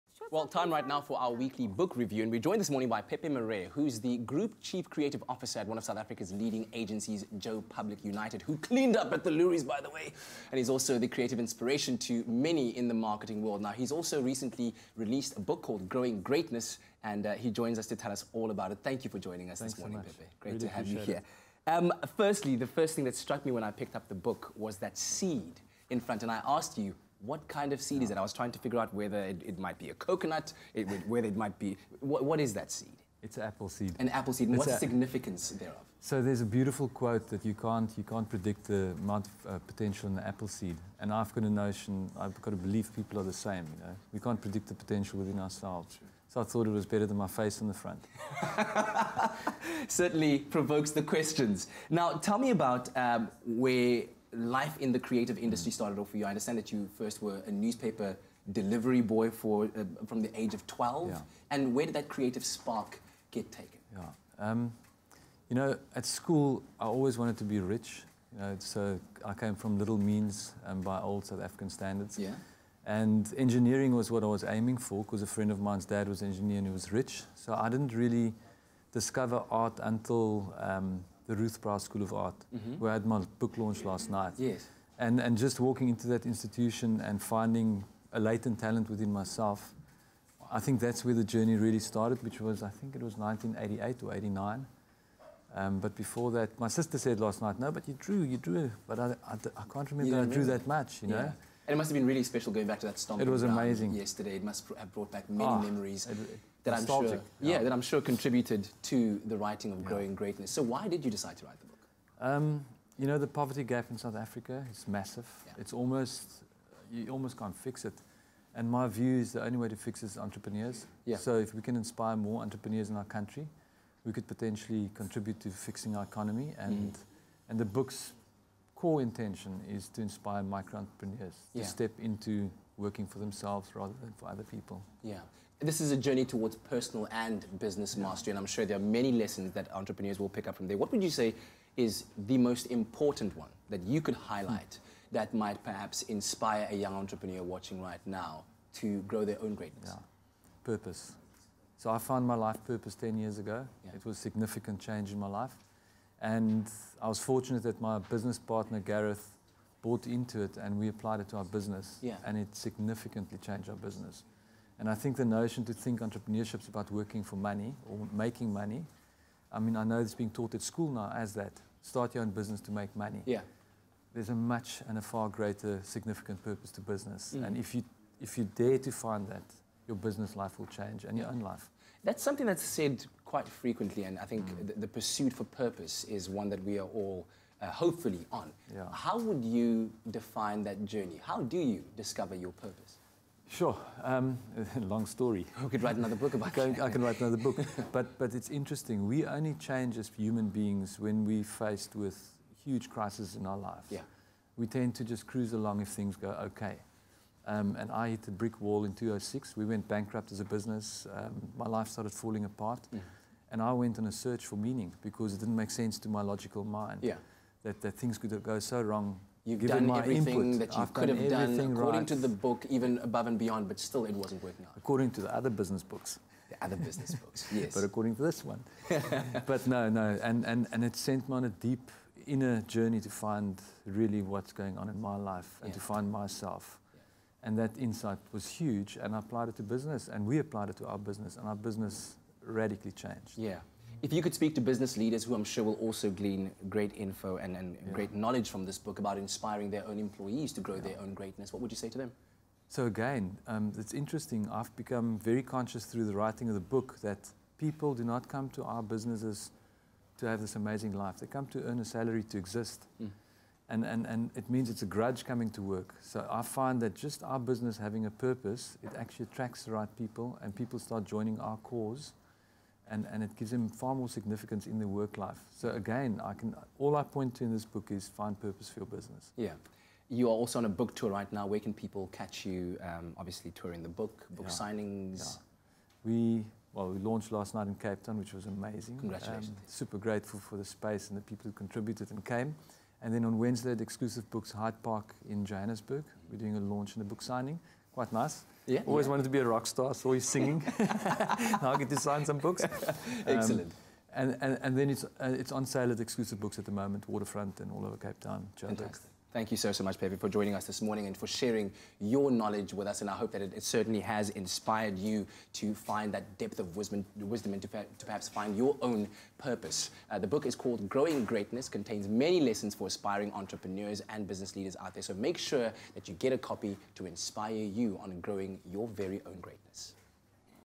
Book Review: 'Growing Greatness' by Pepe Marais
PEPE MARAIS, the author of Growing Greatness, is the founding partner and Group Chief Creative Officer of brand and communications group Joe Public United. He lives for creativity that deeply connects with people.
His passion is to co-create an environment and culture that breed exponential growth – the growth of Joe Public’s people, clients and ultimately our country.